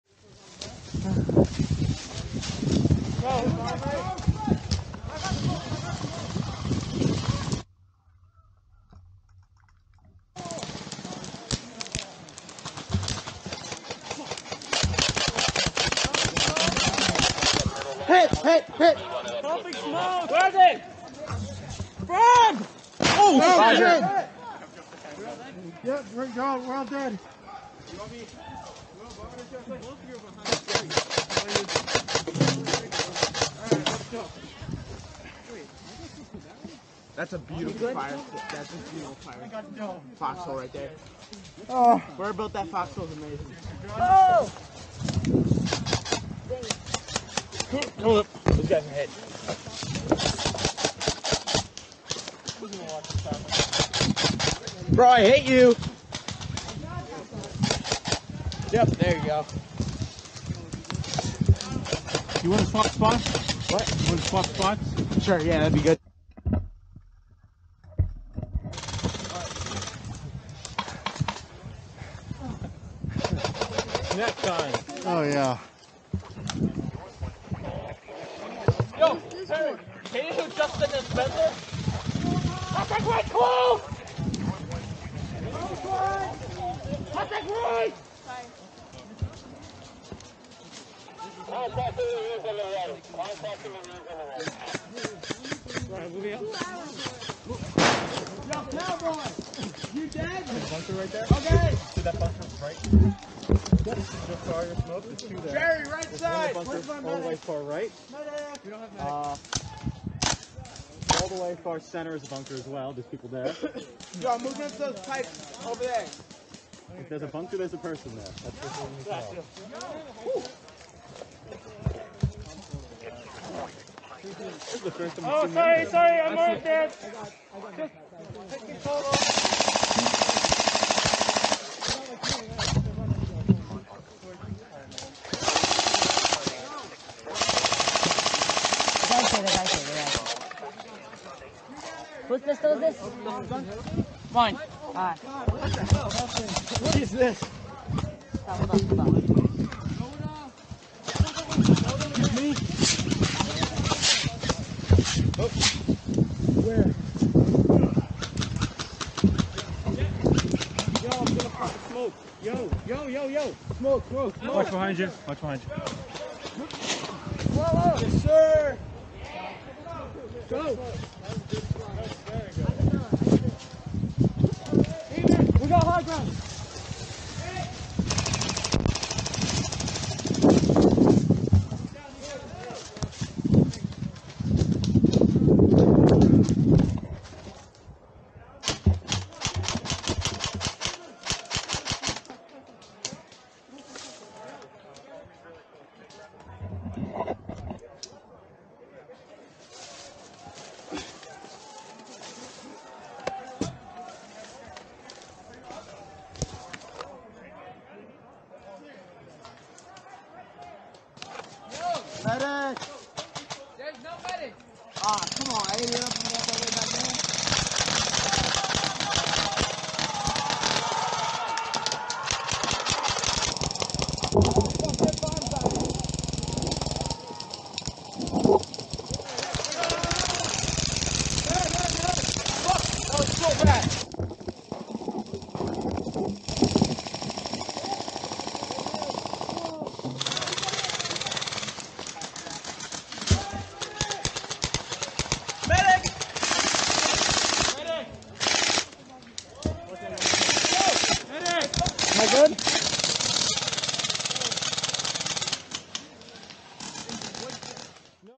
Taking the Hill at Crimson Skies 9
After pushing off from the fort and into the woods, the attacking force must now take the hill overlooking the defending team's final control point.

Event
Field
Camera